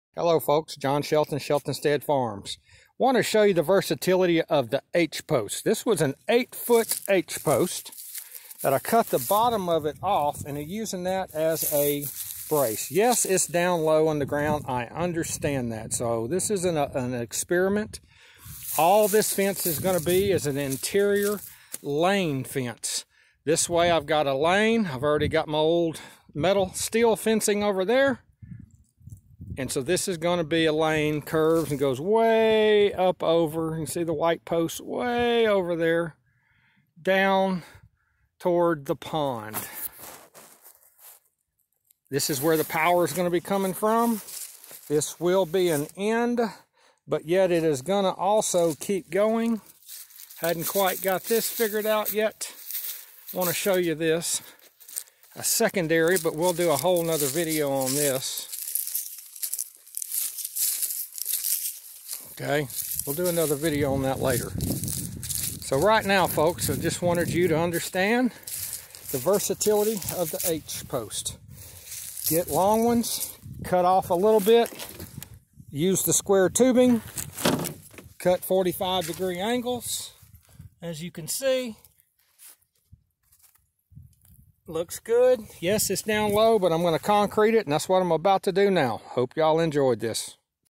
Timeless fence H-post uses...
Making a end brace for a short interior lane fence, out of a 7' Timeless H-post.
I cut off and 45° about 2ft. buried the H-post 18" deep and used 6-8 shovel loads of concrete. I then attached the short cut off part using a 6-7" square tubing also from Timeless fence.
drilled the holes and attached with stainless steel bolts.
This fence is just a short 2-3 strand (not very tight) interior lane fence.
I am NOT spending the money for a A-Frame End brace for this short fence, because it is not needed.